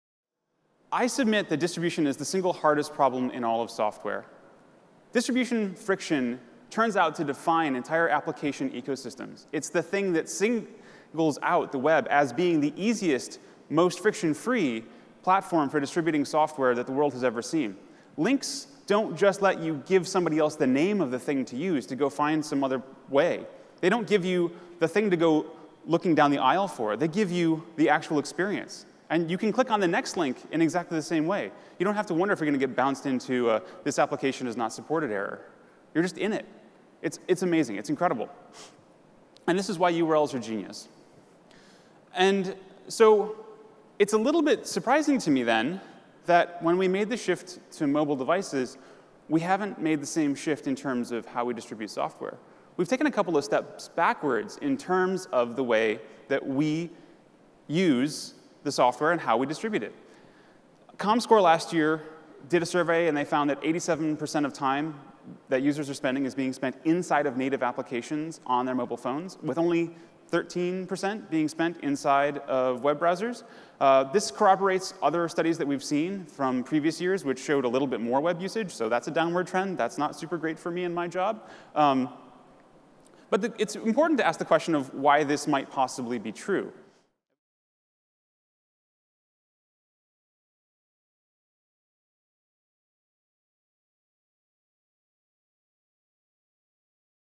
Progressive Web Apps and What's Next for Mobile - Alex Russell (Google)
Web apps have long been considered underpowered compared to native mobile apps, but what's really at the root of this perception? Alex Russell describes a new way to build that combines emerging browser features into something that just might answer the question.